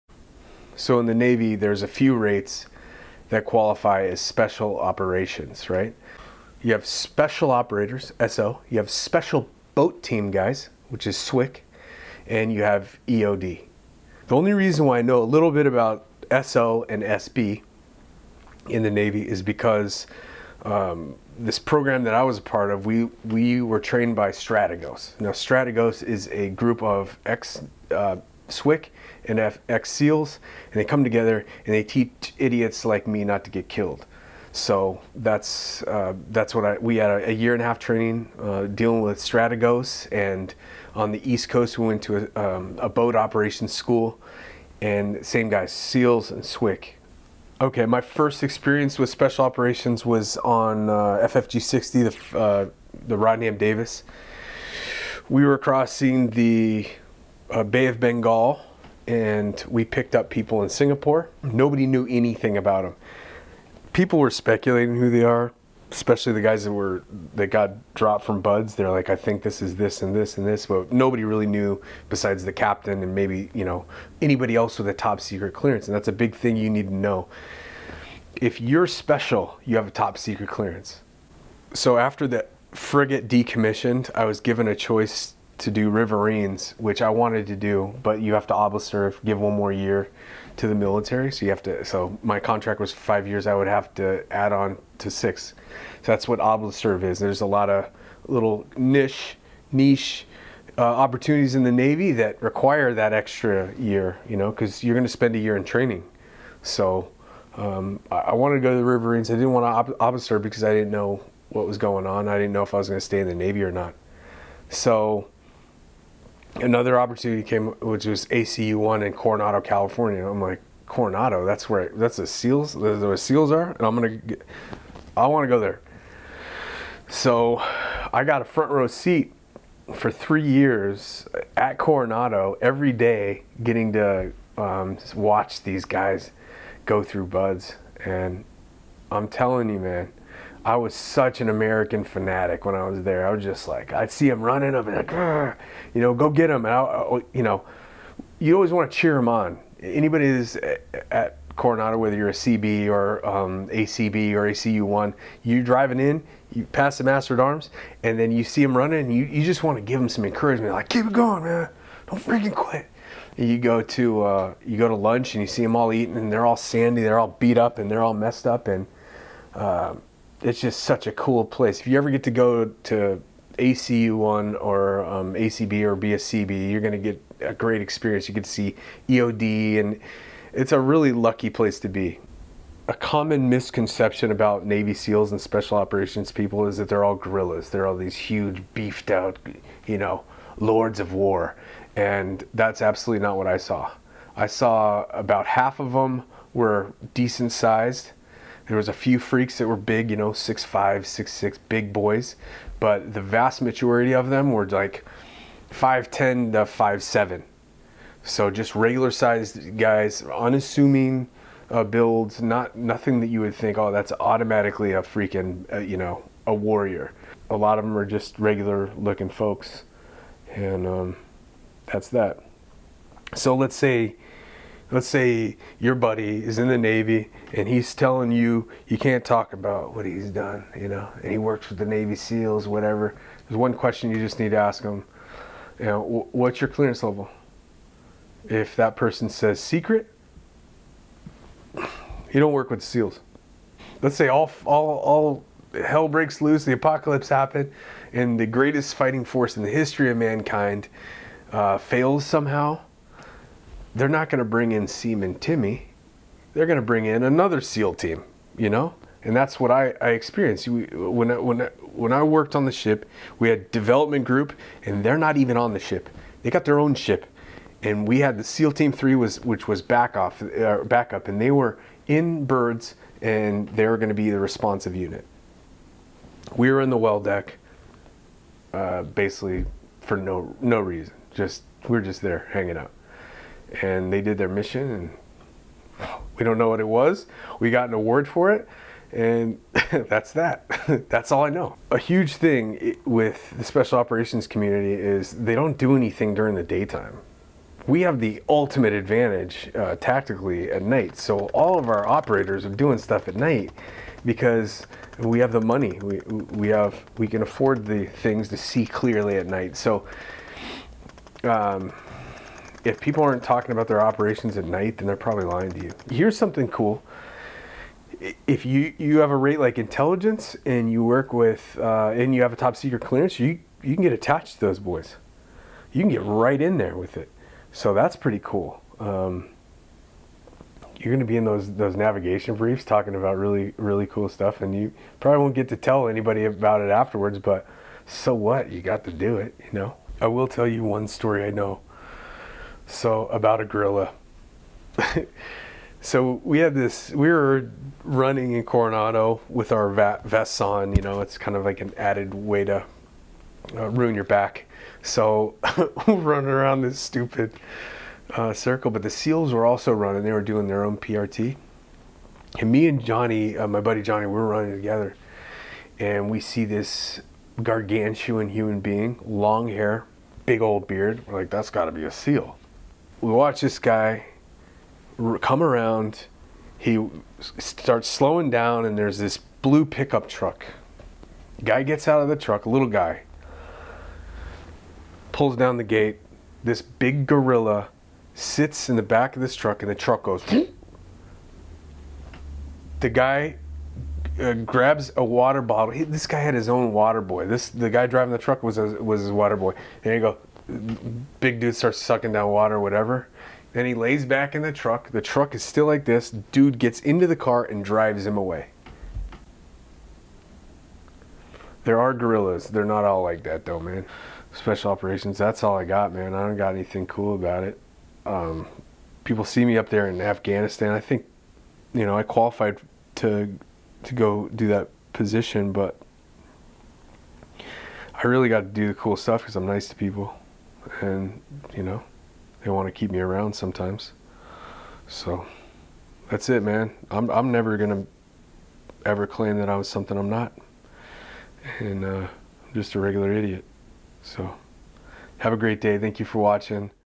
Navy Special Operations & My Experience at Coronado
Breakdown of Navy Special Operations and my experiences during three years at Naval Amphibious Base Coronado.